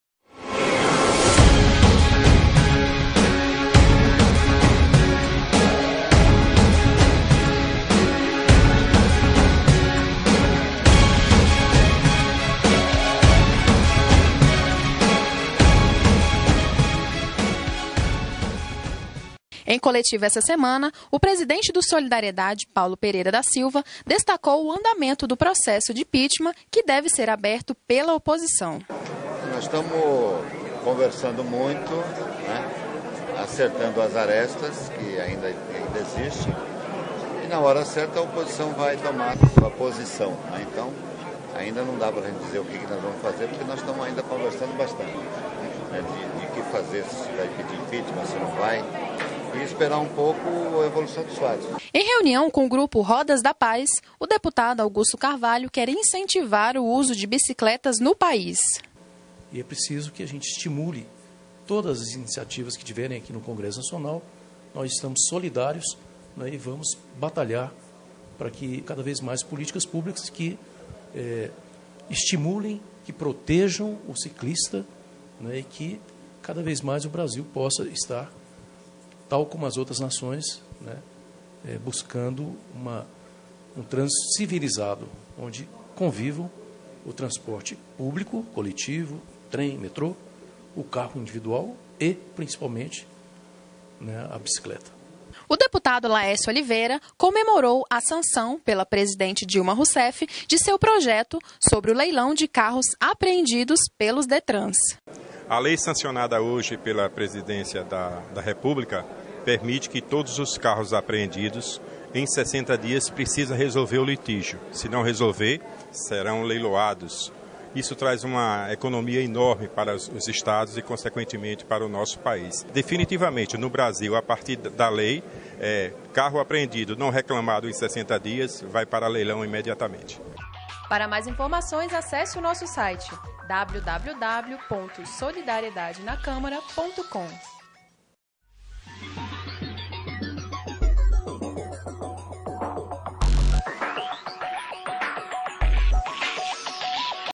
Solidariedade Na Câmara - Fatos da Semana
Impeachment, incentivo ao uso de bicileta, leilão de veículos apreendidos: veja o que foi destaque dos nossos parlamentares esta semana na Câmara.